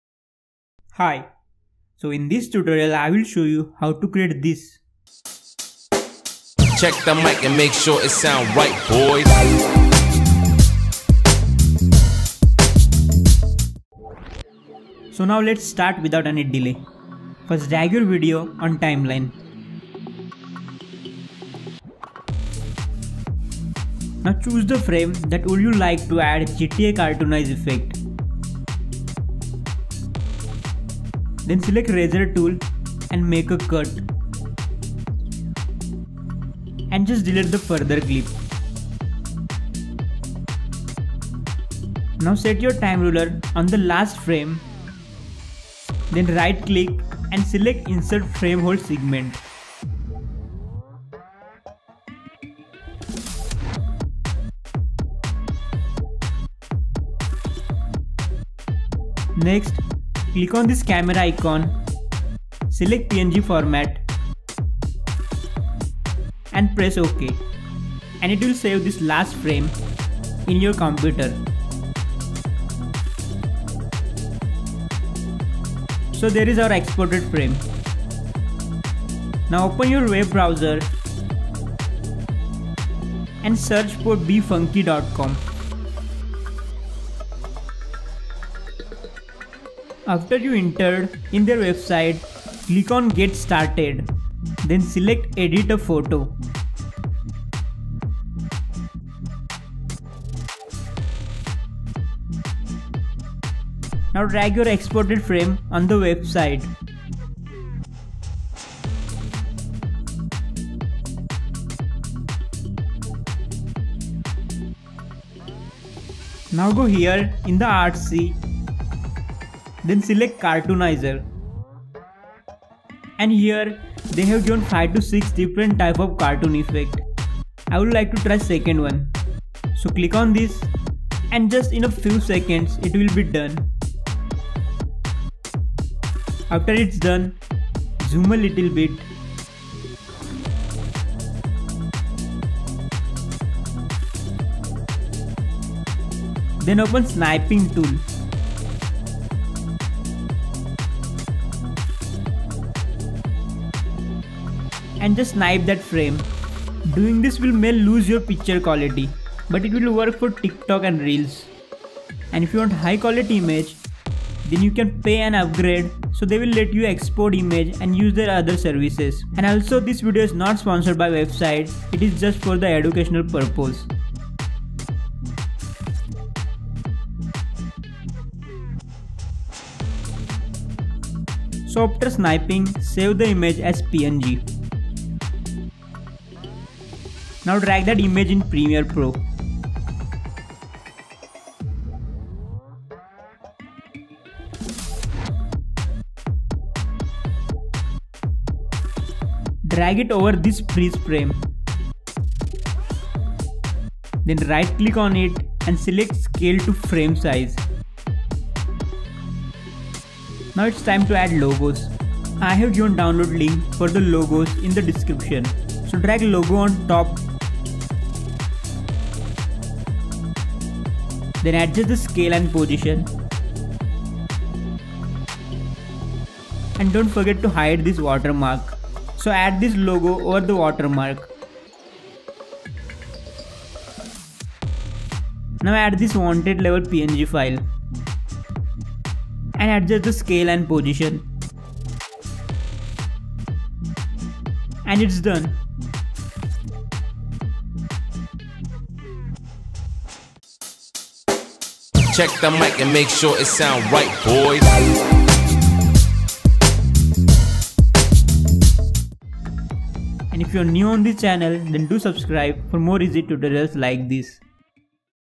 How To Make Trending GTA CARTOONIZE TikTok & Reels | Adobe Premiere Pro ( tutorial )
How How To Do GTA CARTOONIZE Character TikTok & Reels Trend Effect In  Premiere Pro

Hye

My Name Is Tushar And I Will Show You How To Edit Video Easily And How To Do Amazing Effects In Very Easiest Way.